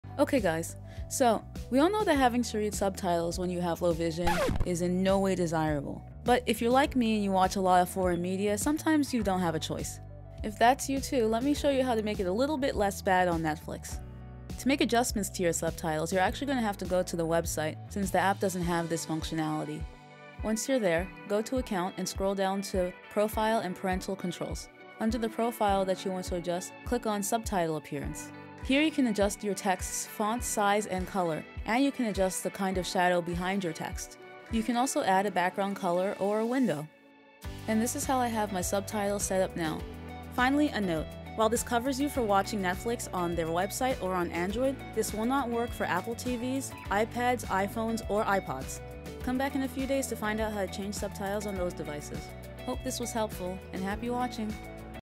How to adjust Netflix subtitles
Ever wonder if you can change subtitles on Netflix ? Here how

Account - Profile and Parental Controls - subtitle appearance

Come back in a few day to find out how to change them on TV OS and iOS products.

Accessibility [visual description]

Scene 1:  Show with normal subtitles playing on laptop
Scene 2: scroll through Netflix catalog on laptop
Scene 3: walk though of subtitle options
Scene 4: show with enlarged subtitle playing on laptop
Scene 5: open Netflix on iPad

👩🏼 Who I am:
Hi friends! I make YouTube videos focused on tech and productivity through the lens of someone who is visually impaired.